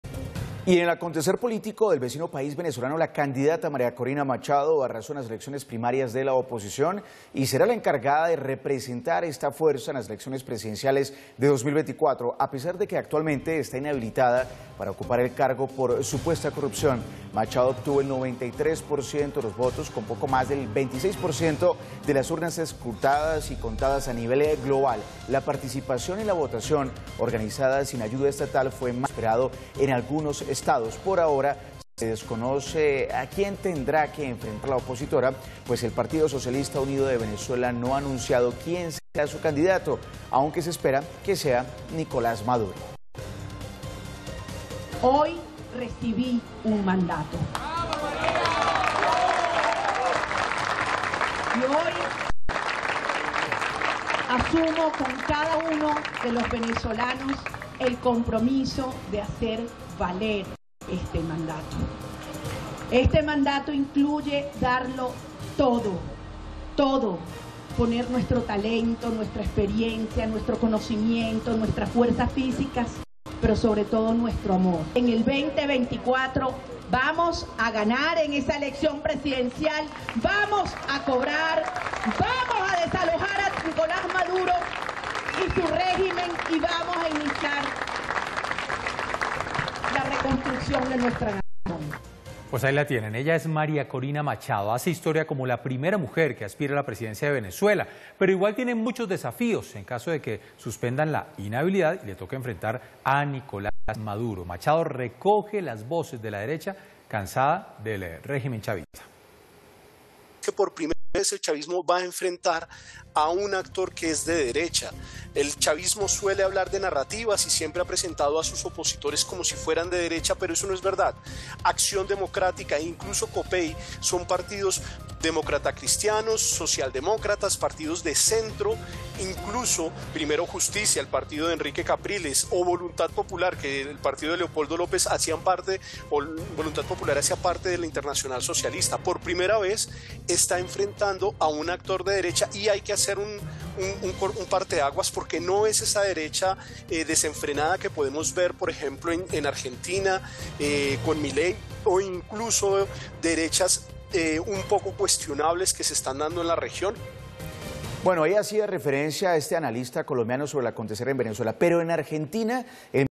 María Machado, la candidata que representaría a la oposición venezolana
Ronal Rodríguez, investigador del Observatorio de Venezuela de la Facultad de Estudios Internacionales, Políticos y Urbanos de la Universidad del Rosario, analiza para Red Más Noticias la victoria de la candidata venezolana en las elecciones primarias de esa nación.